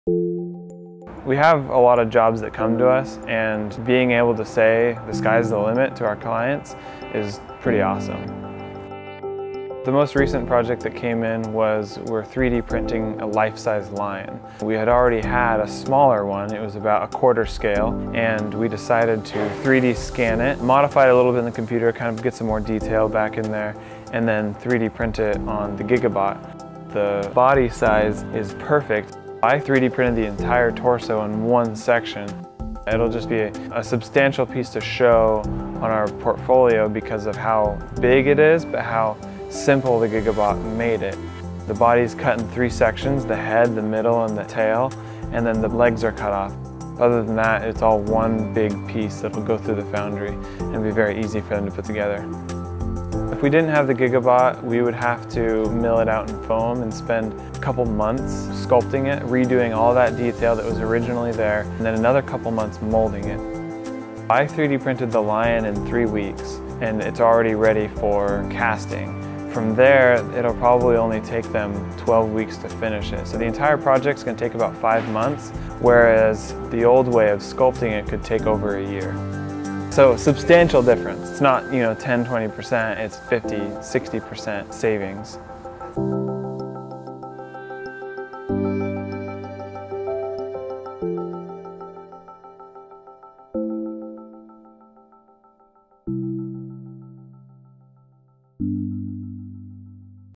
Making a Life Size Bronze Lion
Firebird 3D isn't kidding when they tell their clients that the sky is the limit. Their latest project is to 3D print a life size lion for bronze casting, which they're doing with the help of their Gigabot and cutting costs by up to 60% in the process.